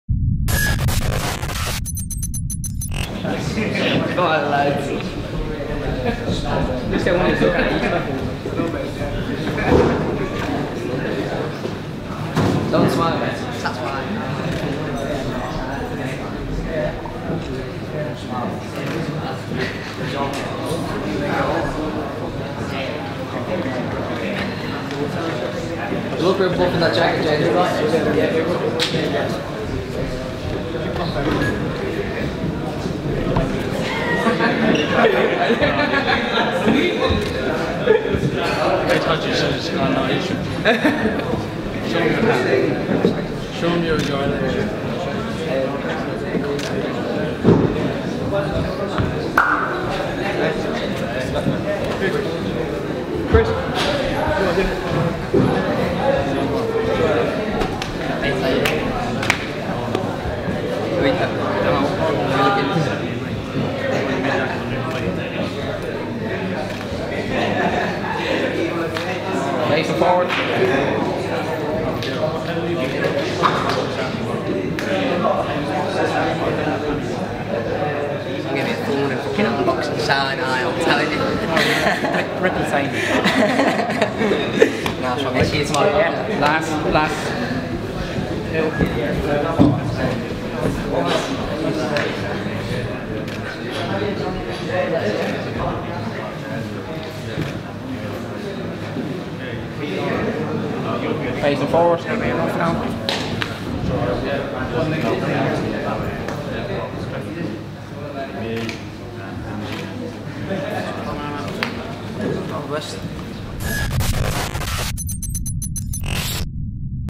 Last Man Standing Face Offs
Last Man Standing-tournament fighters facing off before tomorrow's anticipated event here in Dublin